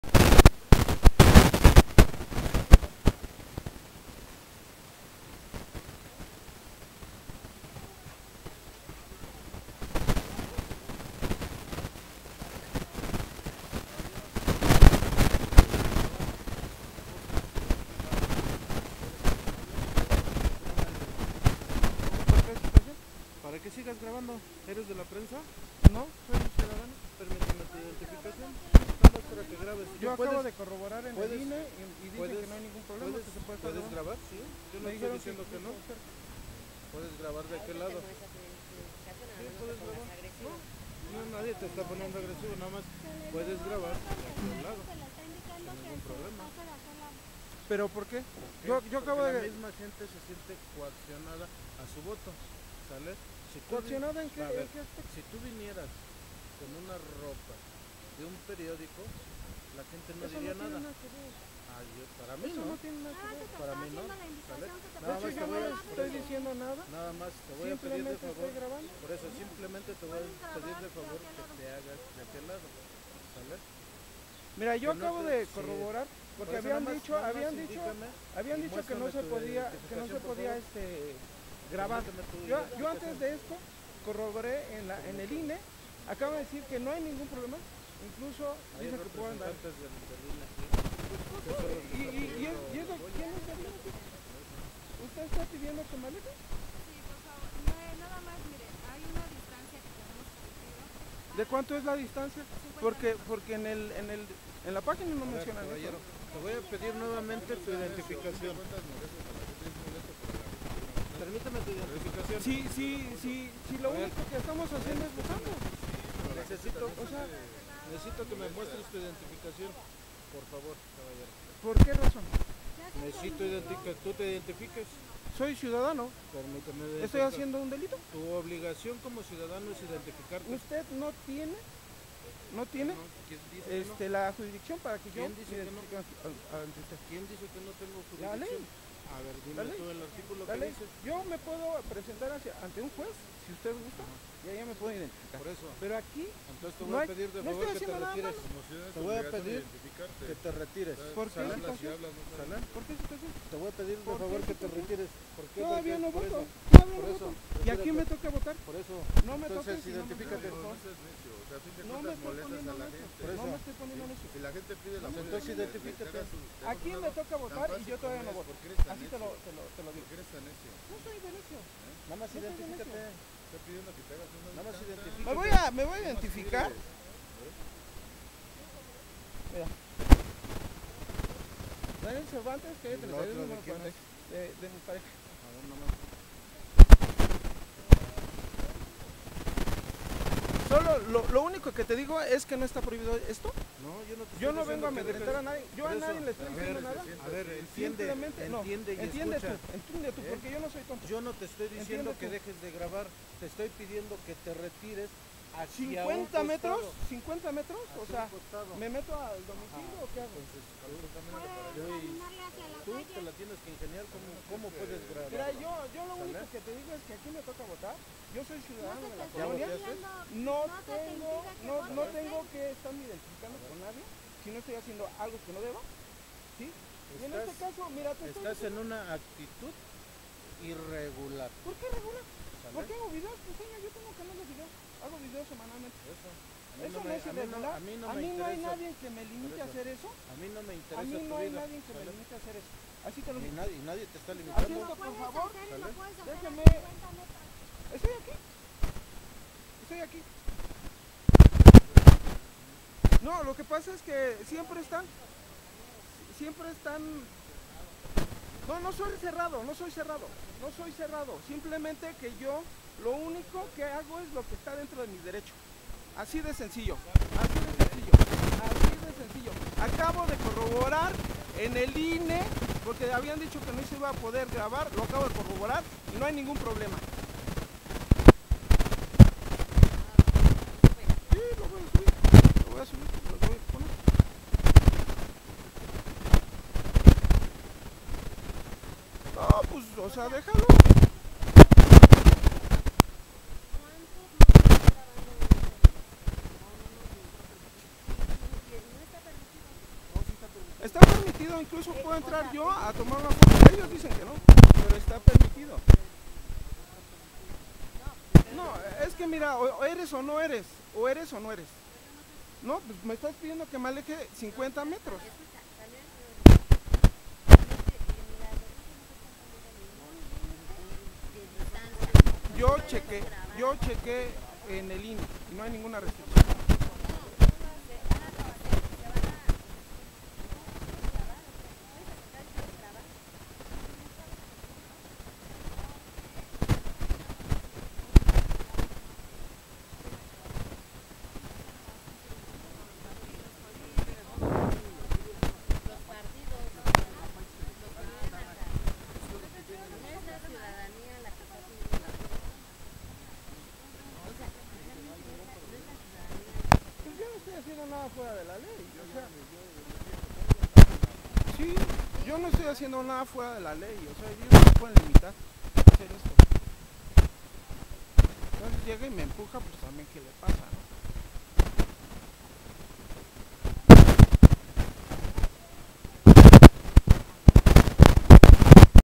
Oficiales me agreden tratan de impedirme grabar en casillas electorales
ejerciendo mi derecho a grabar y dejar recuerdo de este día histórico para mexico, policías tratan de impedirlo, por que según ellos la gente se sentía cuaccionada para emitir  su voto.